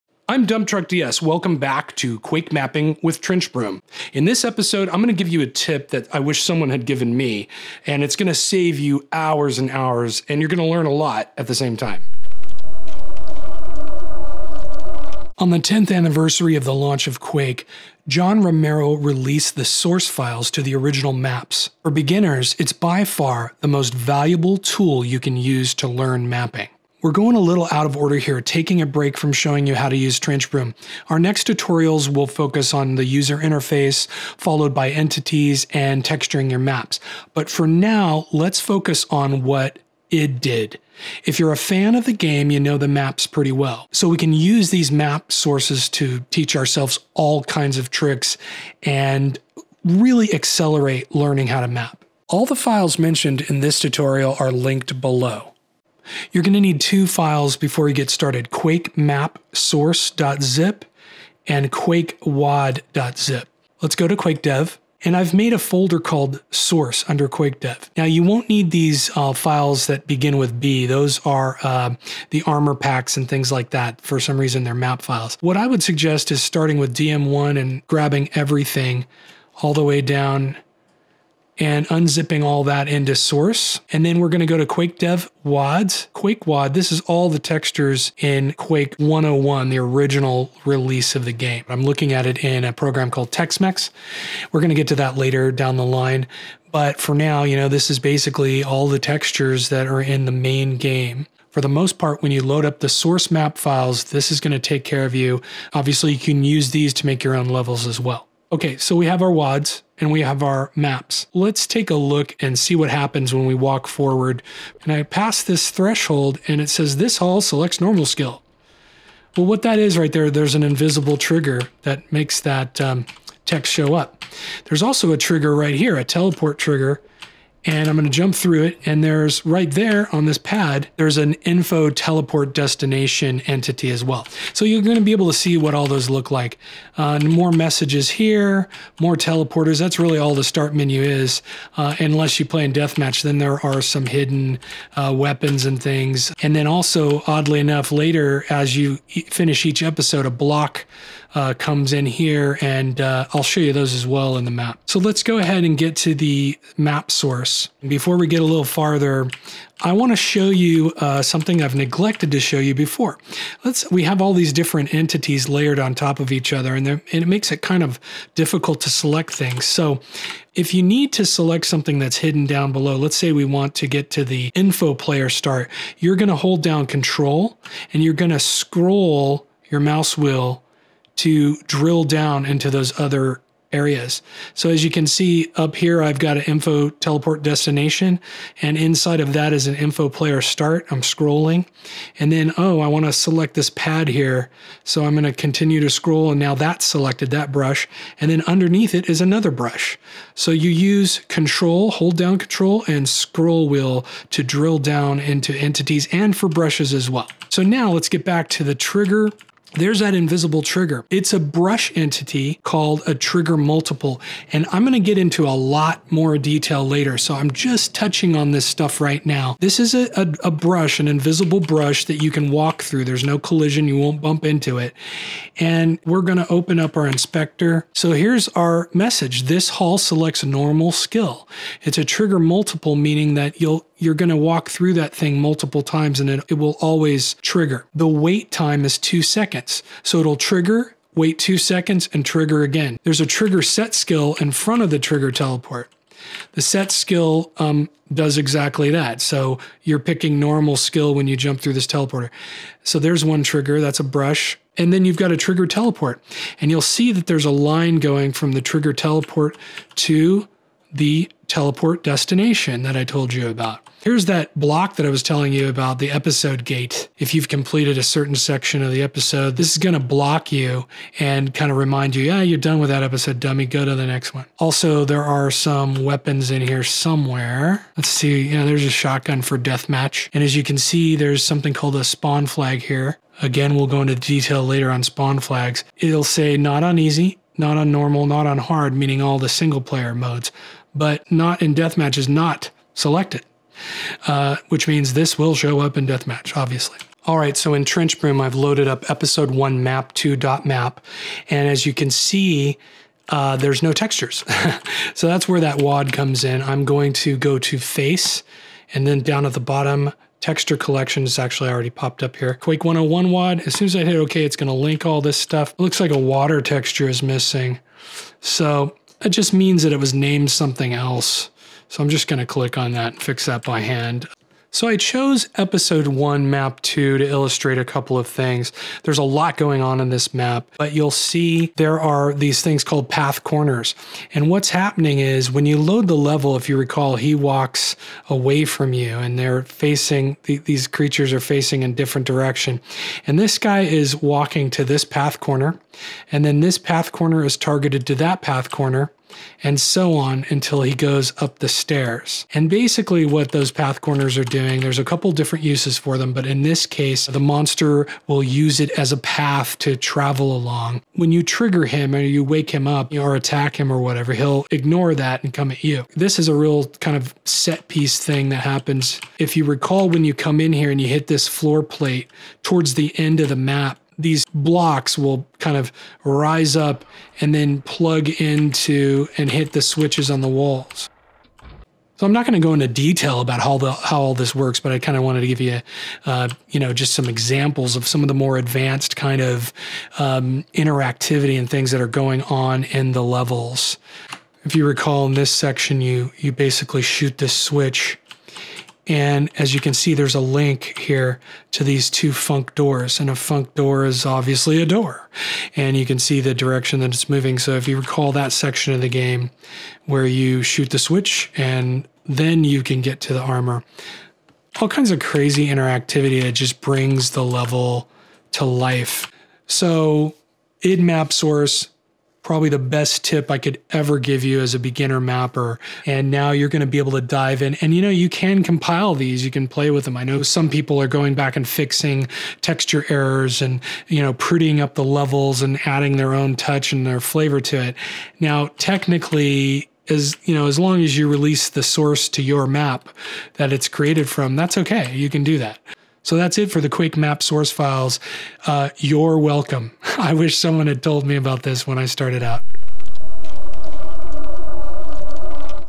Quake Mapping: The id Maps
***UPDATE*** If you have trouble loading the original map files in newer versions of TrenchBroom, select Standard or Auto Detect in the Map Format drop down. This will fix the error! ***

PLEASE NOTE: TrenchBroom has an excellent manual. You can find it under the “Help” menu.

In this episode, see how the original map files can help you teach yourself mapping by examining “what id did.”

Highlights:

0:55 Files Needed
1:31 Quake101.wad
1:51 Start Map
2:39 Drill Down through Selections
3:27 Start.map Source
3:39 trigger_multiple
4:02 trigger_setskill
4:14 trigger_teleport
4:23 func_episodegate
4:34 spawnflags (deathmatch only)
4:57 E1M2.map source
5:31 path_corner
6:06 door unlock set piece
6:34 func_door (yellow armor)